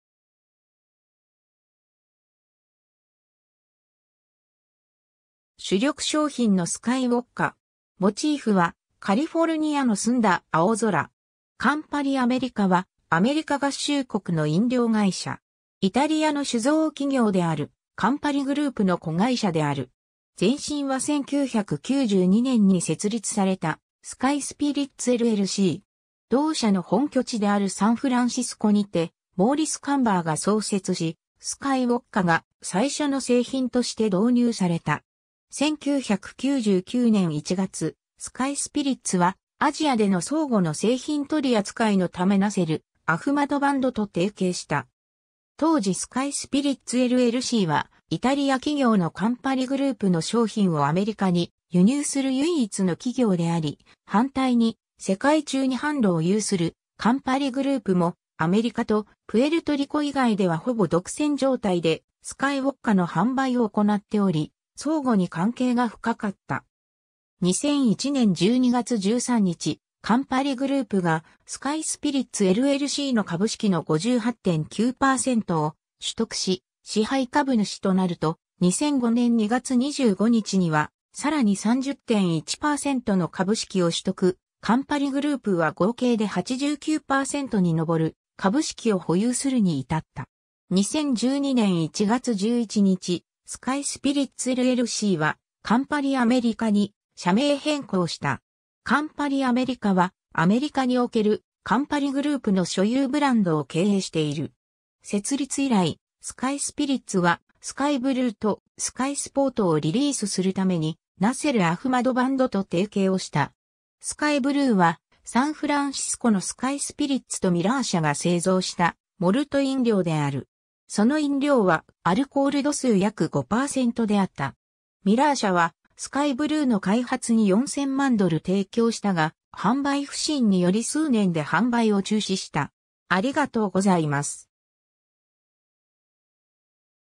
カンパリアメリカ
主力商品のスカイウォッカ。
モチーフはカリフォルニアの澄んだ青空。
 カンパリアメリカ（英語: Campari America）は、アメリカ合衆国の飲料会社。
イタリアの酒造企業であるカンパリグループ（ダヴィデ・カンパリ・ミラノ）の子会社である。
 前身は1992年に設立されたスカイスピリッツLLC。
同社の本拠地であるサンフランシスコにてモーリス・カンバーが創設し、スカイウォッカが最初の製品として導入された。
1999年1月、スカイスピリッツは、アジアでの相互の製品取り扱いのためナセル・アフマドヴァンドと提携した。
当時スカイスピリッツLLCはイタリア企業のカンパリグループの商品をアメリカに輸入する唯一の企業であり、反対に世界中に販路を有するカンパリグループも、アメリカとプエルトリコ以外ではほぼ独占状態でスカイウォッカの販売を行っており相互に関係が深かった。
2001年12月13日、カンパリグループがスカイスピリッツLLCの株式の58.9%を取得し支配株主となると、2005年2月25日にはさらに30.1%の株式を取得、カンパリグループは合計で89%に上る株式を保有するに至った。
2012年1月11日、スカイスピリッツLLCはカンパリアメリカに社名変更した。
 カンパリアメリカは、アメリカにおけるカンパリグループの所有ブランドを経営している。
 設立以来、スカイスピリッツは、スカイブルーと、スカイスポート（両方ともモルト・リキュール飲料）をリリースするためにナセル・アフマドヴァンド (Nasser Ahmadvand) と提携をした。
 スカイブルー（SKYY Blue）は、サンフランシスコのスカイスピリッツとミラー社（Miller Brewing Company）が製造したモルト飲料である。
その飲料はアルコール度数約5%であった。
ミラー社はスカイブルーの開発に4千万ドル提供したが、販売不振により数年で販売を中止した。